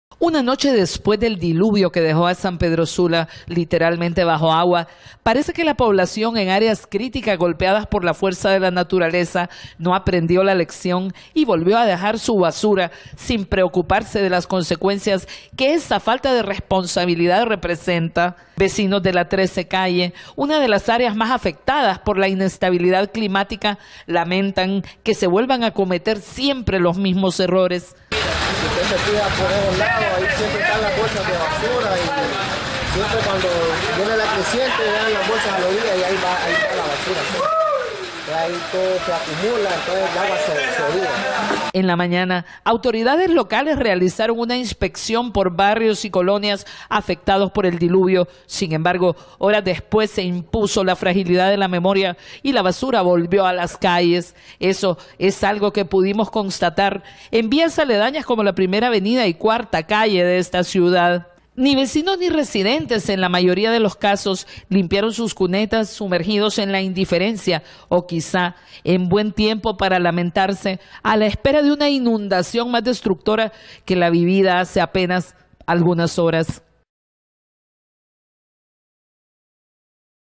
Sampedranos a la esperan de una inundación más destructora
Una noche después del diluvio que dejó a San Pedro Sula literalmente bajo agua, parece que la población en áreas críticas golpeadas por la fuerza de la naturaleza, no aprendió la lección y volvió a dejar su basura sin preocuparse de las consecuencias que esa falta de responsabilidad representa.